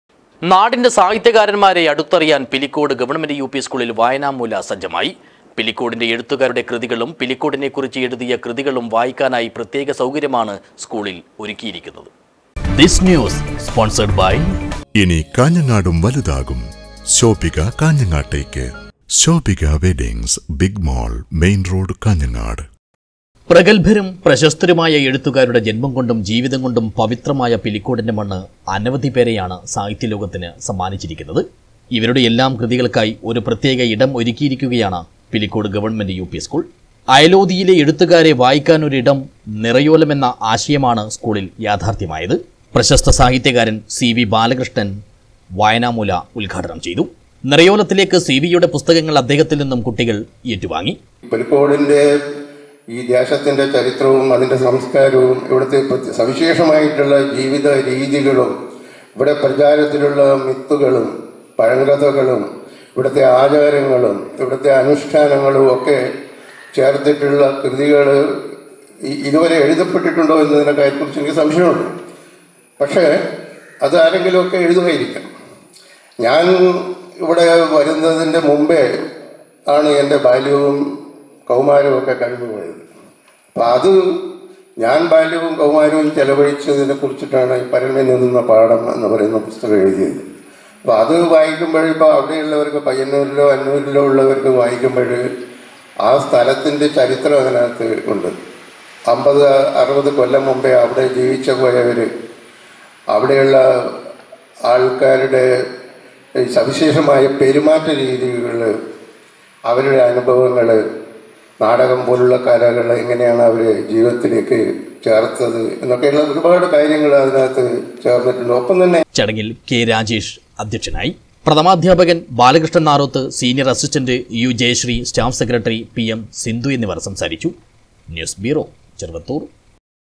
നാടിന്റെ എഴുത്തുകാരെ അടുത്തറിയാന്‍ പിലിക്കോട് ഗവ:യുപി സ്‌കൂളില്‍ വായനാമൂല സജ്ജമായി.
നാടിന്റെ എഴുത്തുകാരെ അടുത്തറിയാന്‍ പിലിക്കോട് ഗവ:യുപി സ്‌കൂളില്‍ വായനാമൂല സജ്ജമായി. പിലിക്കോടിന്റെ എഴുത്തുകാരുടെ കൃതികളും പിലിക്കോടിനെക്കറിച്ച് എഴുതിയ കൃതികളും വായിക്കാനായി പ്രത്യേക സൗകര്യമാണ് സ്‌കൂളില്‍ ഒരുക്കിയിരിക്കുന്നത്.